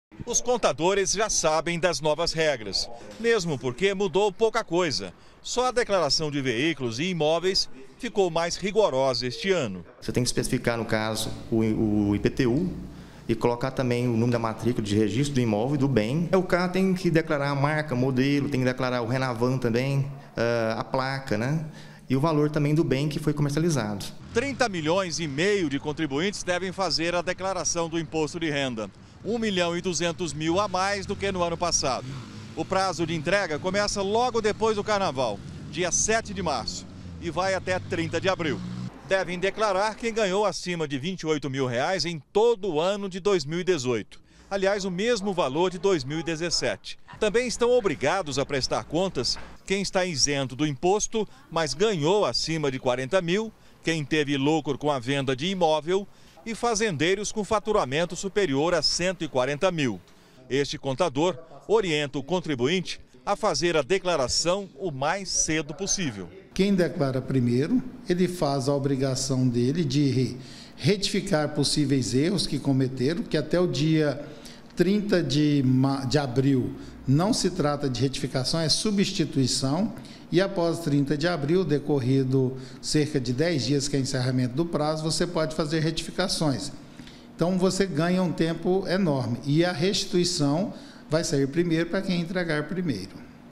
IMPOSTO DE RENDA: DECLARAÇÃO COMEÇA DEPOIS DO CARNAVAL
A gente falou de carnaval o jornal todo. Mas logo depois da festa, tem amolação pela frente. Começa o prazo para entregar a declaração do imposto de renda. A Receita Federal divulgou hoje as regras para a declaração do ano base 2018.